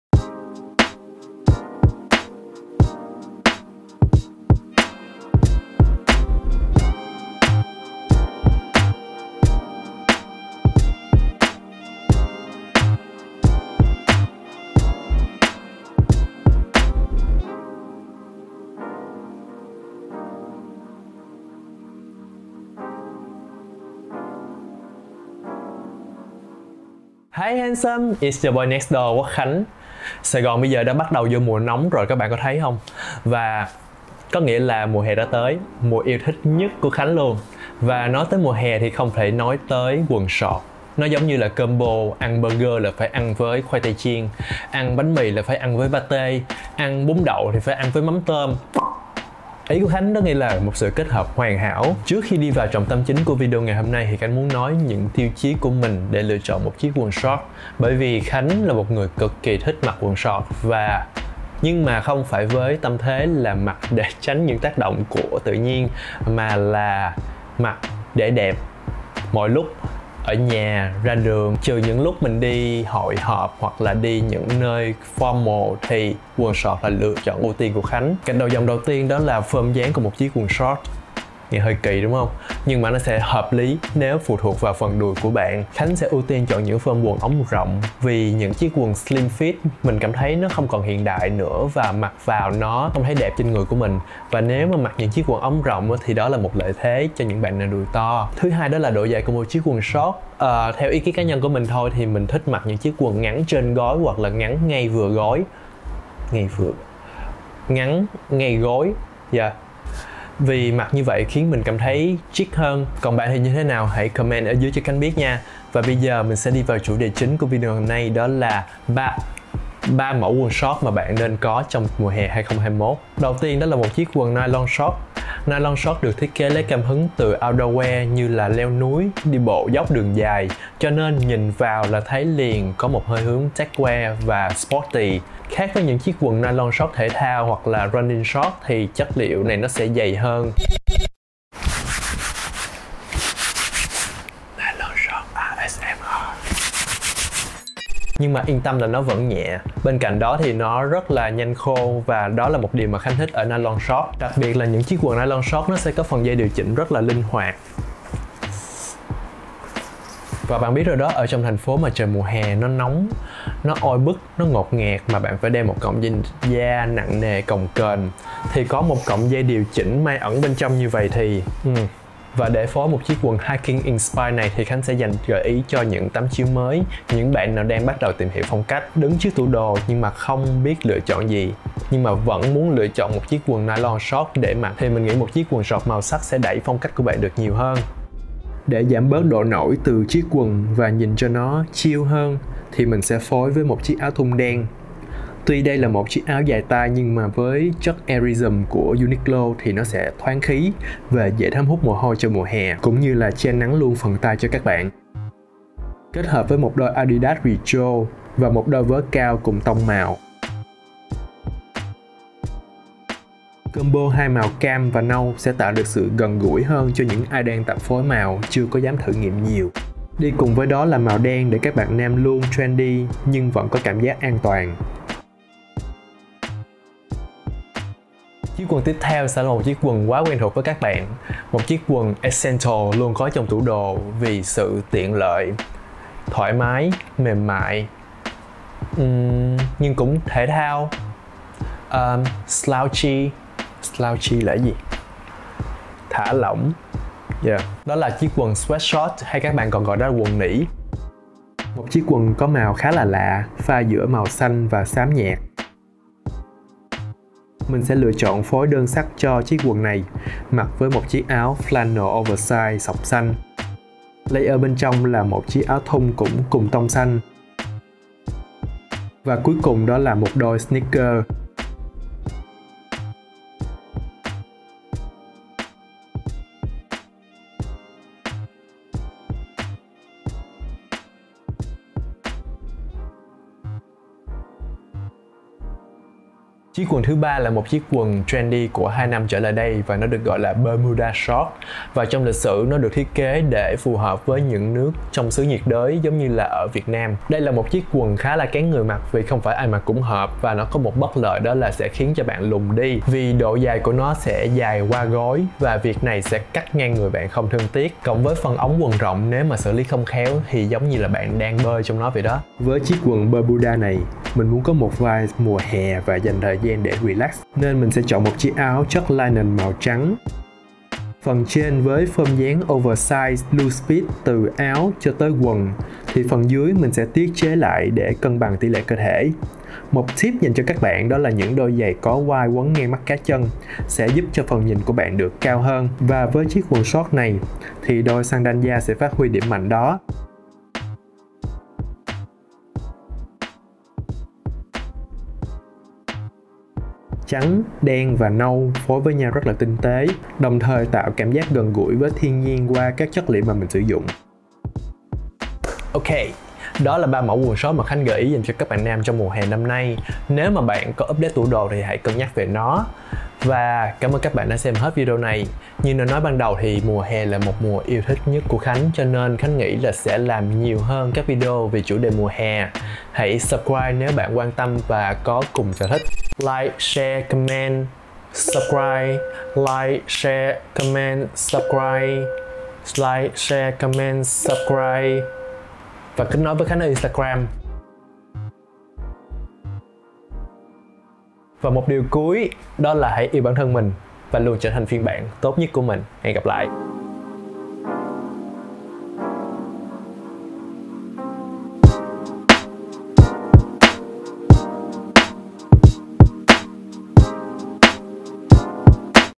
3 chiếc quần shorts nên có cho mùa hè!
T H A N H L Ý Q U Ầ N Á O C Ò N M Ớ I / T Ố T

G Ặ P K H Á N H Ở Đ Â Y

L I N K S K I Ệ N 🔗

Look 1:
- Nylon shorts: UNIQLO
- Long sleeve Tee: Airism UNIQLO
- Sneakers: Adidas
- Vớ: UNIQLO

Look 2:
- Swear shorts: H&M
- Flannel shirts: Zara
- Tank top: UNIQLO
- Sneakers: Nike Air Jordan

Look 3:
- Bermuda shorts: UNIQLO
- White shirts: Zara
- Leather sandals

Ủng hộ mình tại:
🎯 Banking account: Techcombank
STK: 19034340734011 - Hà Mỹ Quốc Khánh